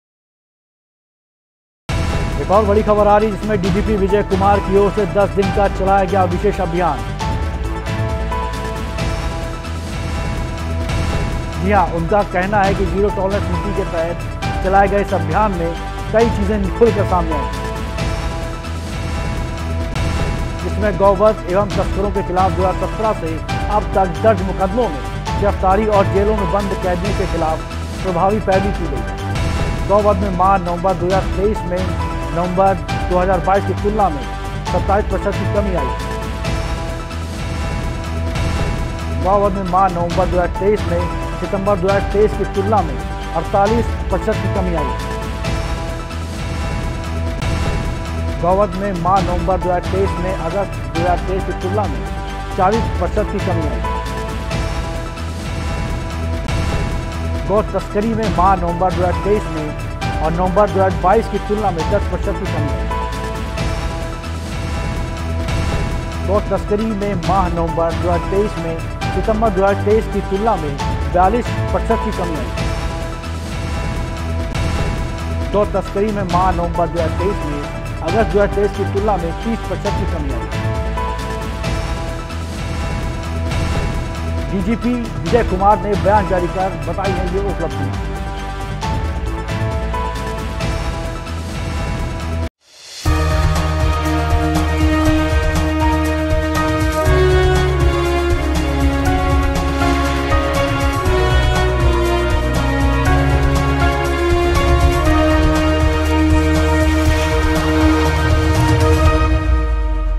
Breaking News : डीजीपी विजय कुमार की ओर से 10 दिन का चलाया गया विशेष अभियान || Tv24 Network
You Tube Channel :   @tv24network